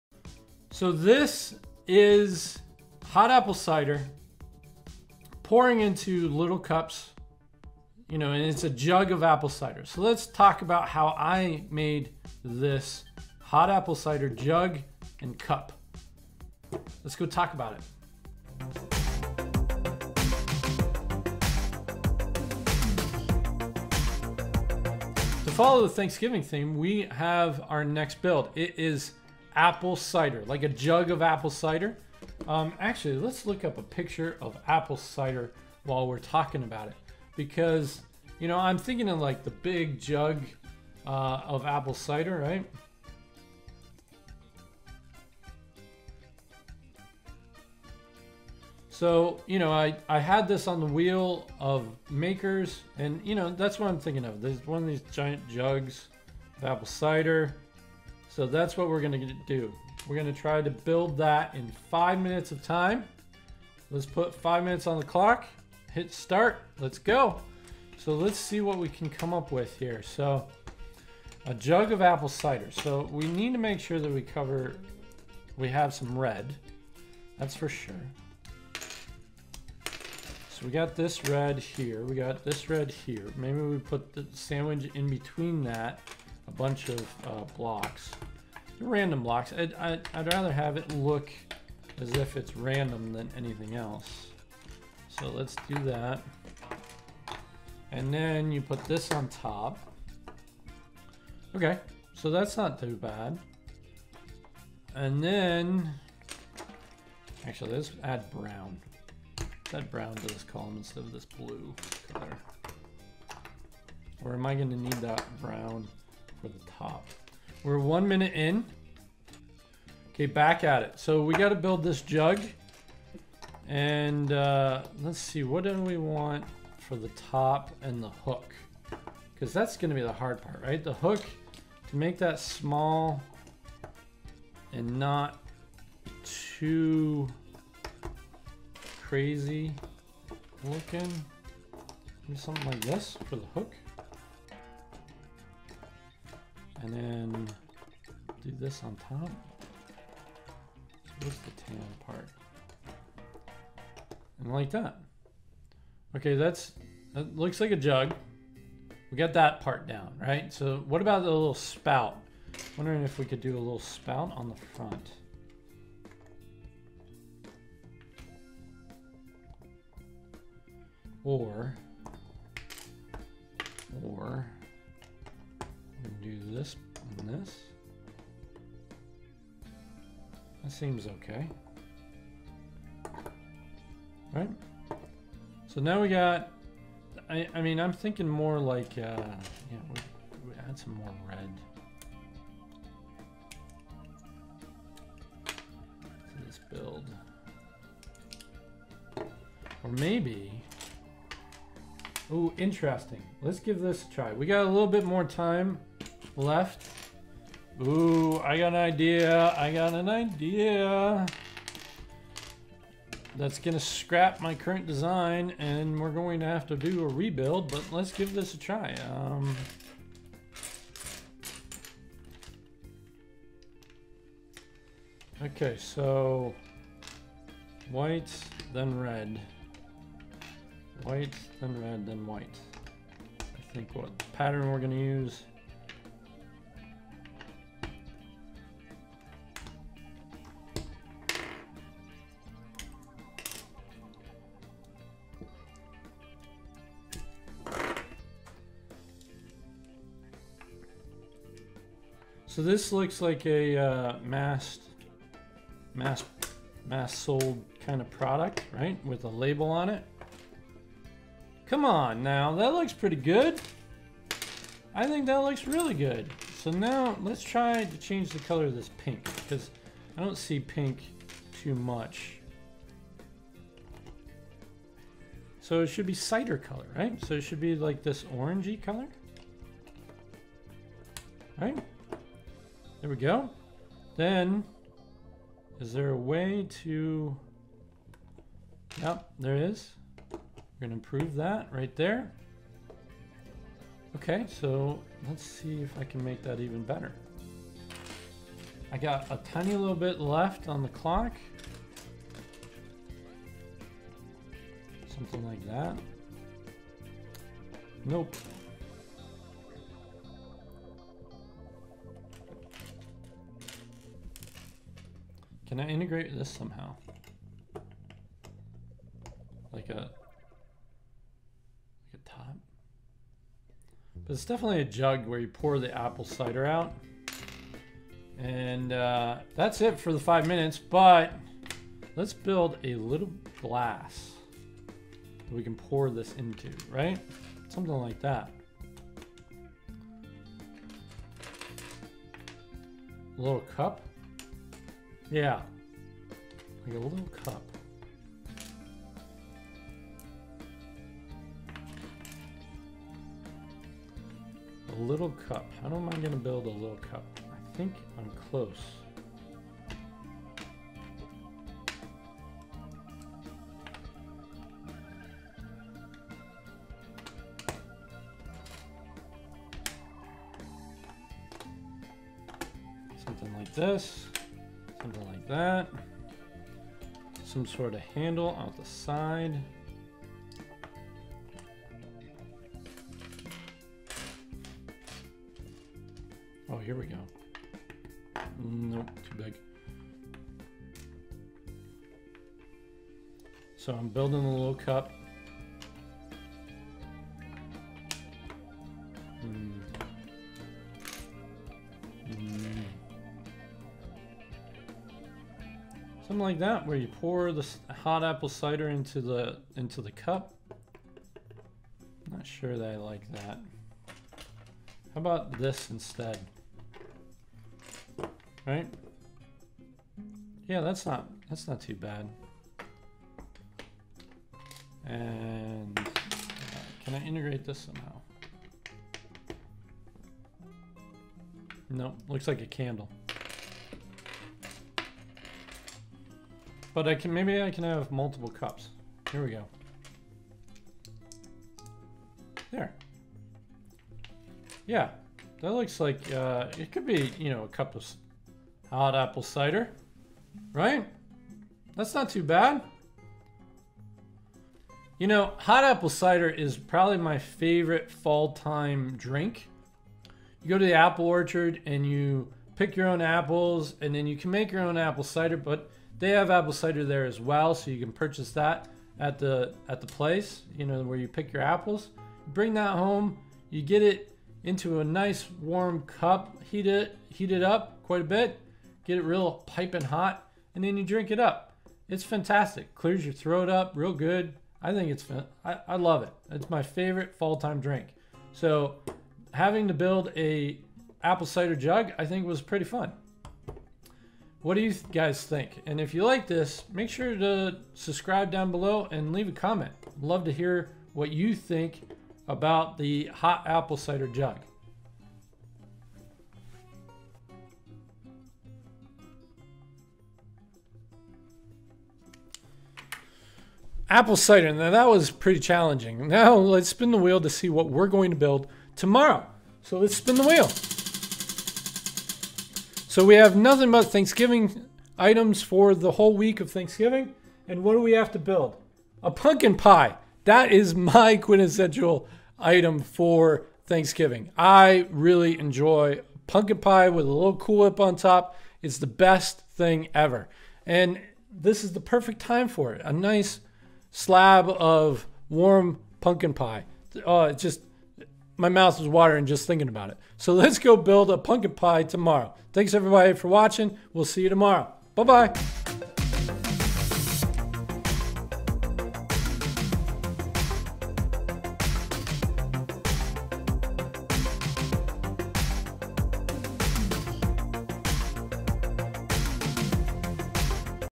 Lego APPLE CIDER - LEGO MOC in 5-Minute Challenge Thanksgiving Week DIY
Lego APPLE CIDER  in 5-Minute Speed Build Challenge Thanksgiving  Week DIY
Please Submit your Creations to our Community:

Will Be doing 10 minute challenge with this Kit